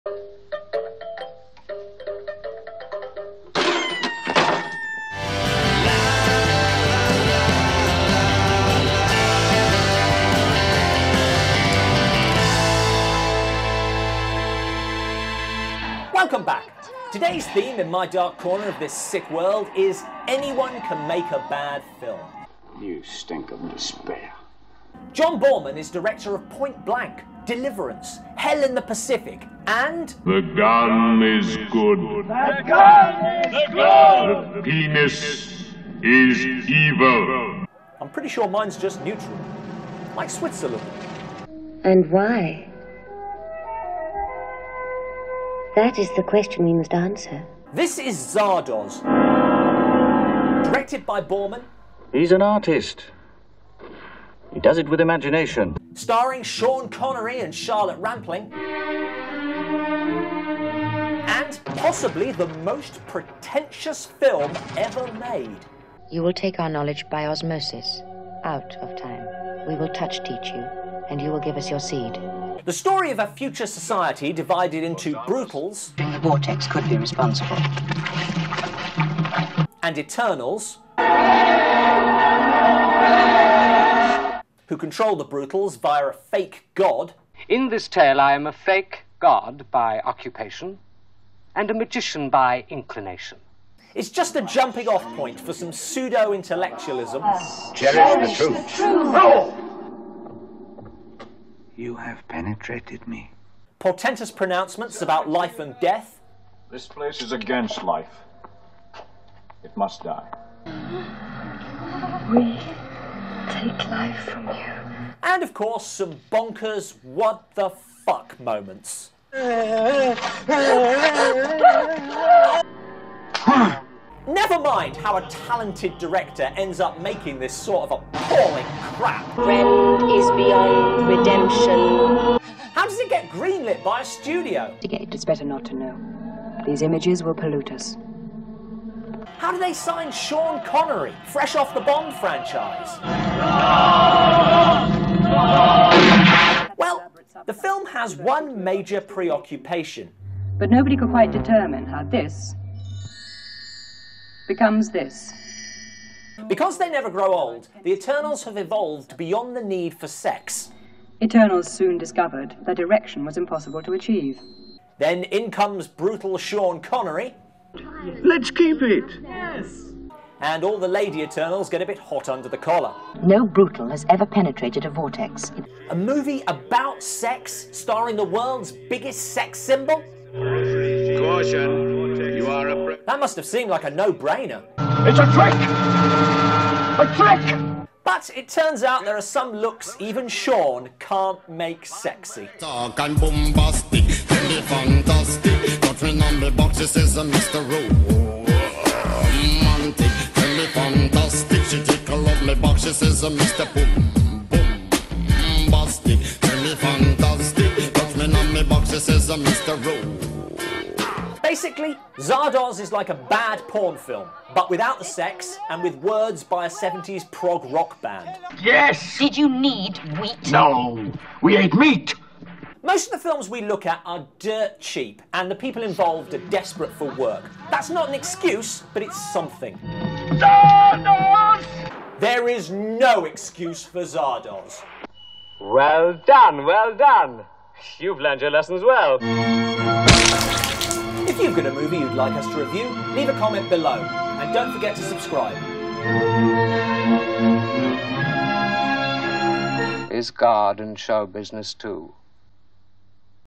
Dark Corners - Zardoz: Review
Beyond 1984, Beyond 2001, Beyond Love, Beyond Death

IMDb Summary: Zed is a exterminator, a savage warrior living in a post-apocalyptic future world. Like all exterminators, Zed worships a stone head called Zardoz as a god. Zardoz promises all those who worship that when they die, they will go to the Vortex, a community inhabited by immortal men and women. Zed finds himself in Zardoz's mouth and Zardoz arrives in the Vortex. Zed's arrival in the Vortex begins to cause disruption, as Zed sets out to discover the secrets of Zardoz. Where Zed encounters a stranger named Arthur Frayn in a library and Arthur teaches Zed to read and reveals Zardoz's secrets, which could bring about the fall of all life in The Vortex and lead Zed to giving the immortals, the gift of death.

Written by & staring Robin Bailes @robinbailes
Directed and Edited by Graham Trelfer @thelcw